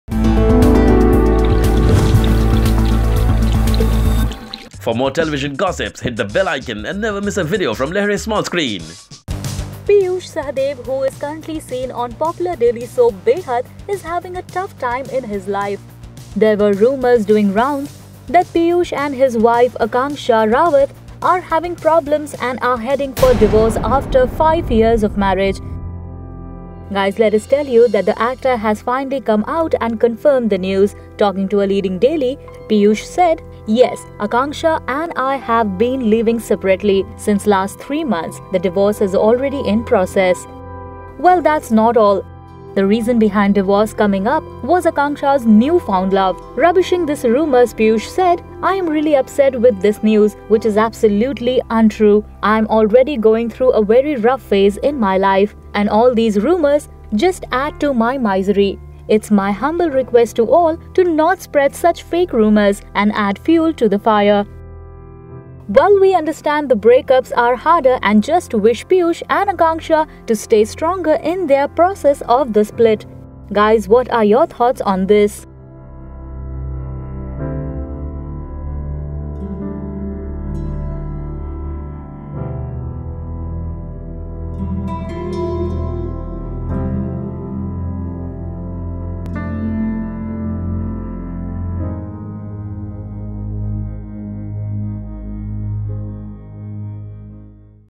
'Beyhadh' Actor Piyush Sahdev CONFIRMS Divorce With Wife Akangsha Rawat
Television actor Piyush Sahdev who made his grand comeback in 'Beyhadh' is going through a bad phase in his real life. Piyush has confirmed that he is heading for a divorce with his wife Akangsha Rawat. To know more check out this video.